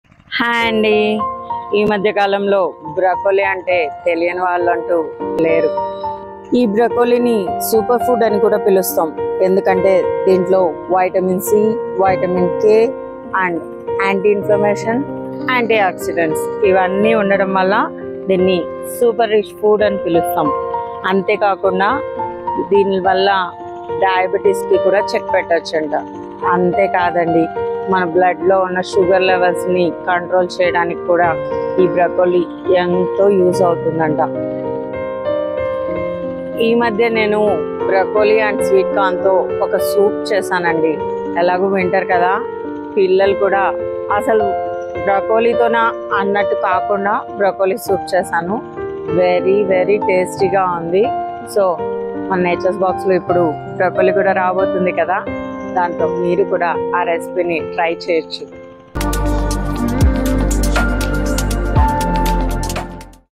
Forget sugar with this green treasure | Easy diabetes control | Health benefits of broccoli
Forget sugar with this green treasure | Easy diabetes control | Health benefits of broccoli | Nature's Box

Want to know the health advantages of this leafy green veggie? Watch the video to understand its benefits and the best way to use it.

For updates on simplified tips for a healthy lifestyle. Connect with kavithasatish on Instagram